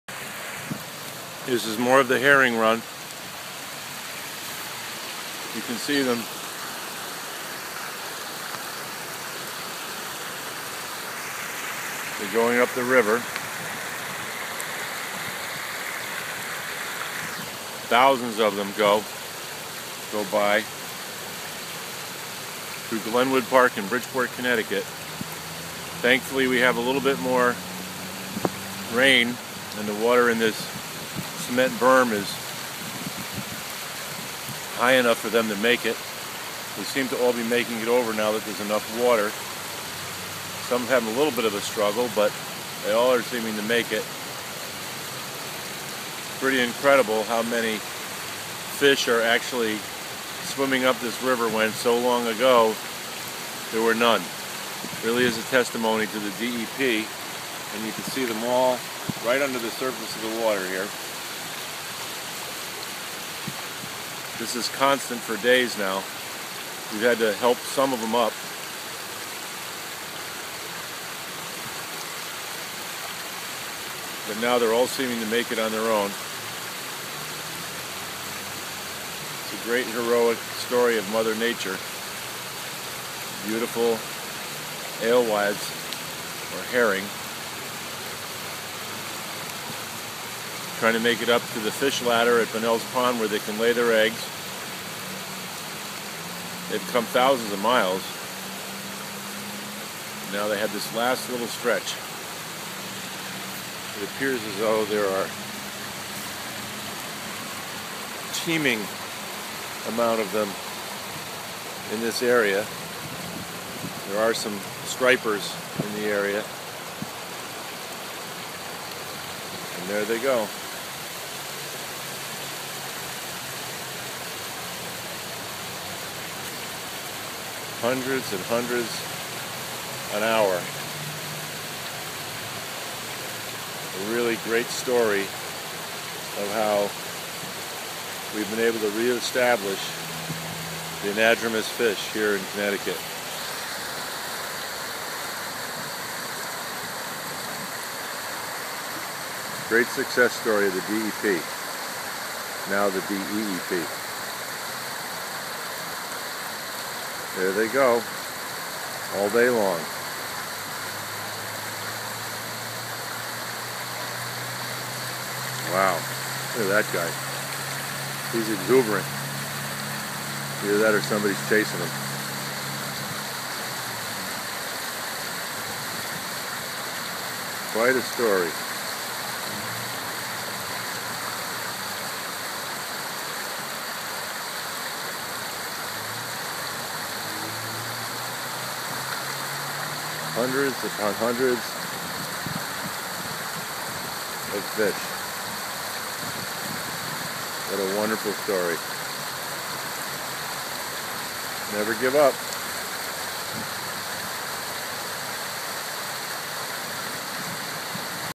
Thousands of migrating fish
Pequonnock River, Bpt, CT